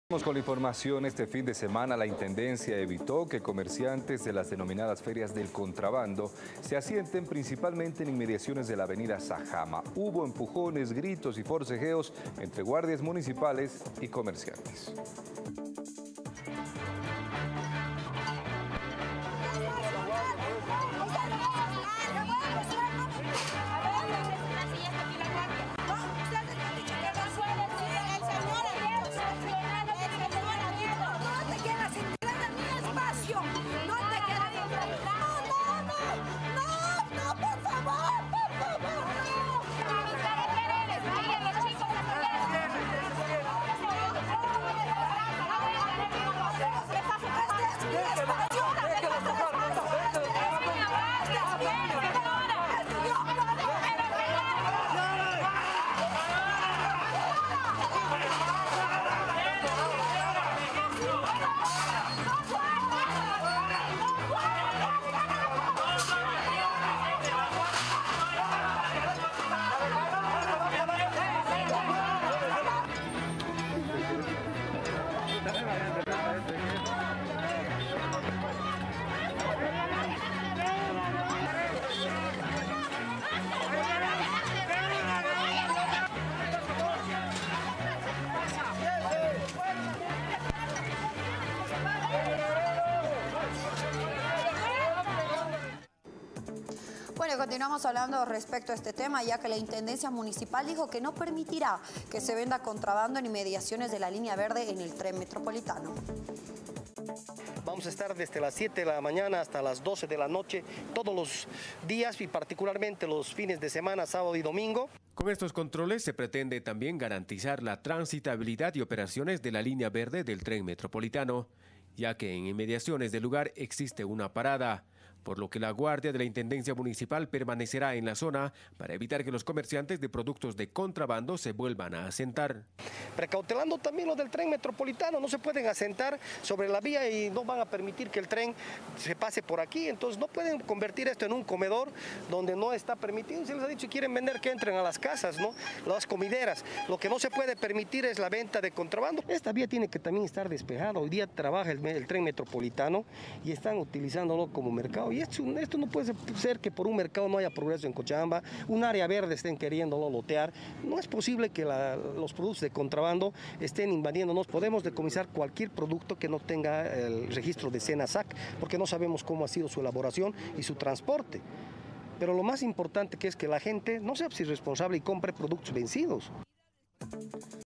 Enfrentamientos entre comerciantes y guardias municipales en Cochabamba
Durante el fin de semana (sábado) la Intendencia Municipal de Cochabamba realizó un operativo de control a comerciantes que forman parte de la denominada feria del contrabando en la avenida Sajama. Entre empujones e insultos, guardias municipales intentaron hacer cumplir su labor, sin embargo, no contaron con la resistencia aguerrida de las personas del comercio, se conoce que al menos 4 personas (guardias) resultaron heridas.